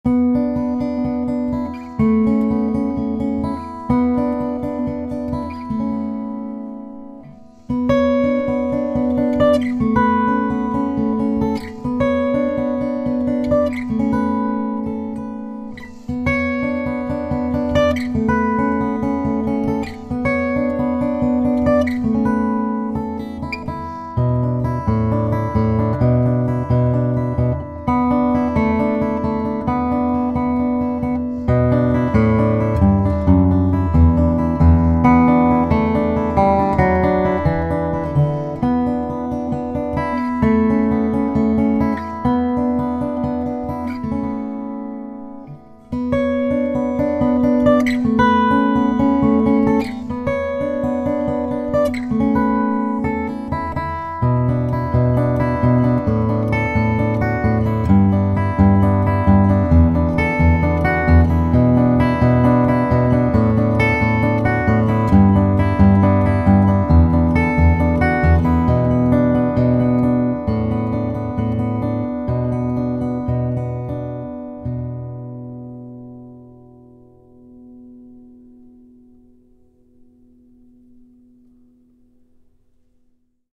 Kostal Modified Dreadnought Acoustic Guitar Played By Will McNicol (Part One)
This recent pre-owned arrival is a powerful, full-toned Modified Dreadnought from Jason Kostal, with a Manzer Wedge for comfort. Here's the one and only Will McNicol demoing the guitar!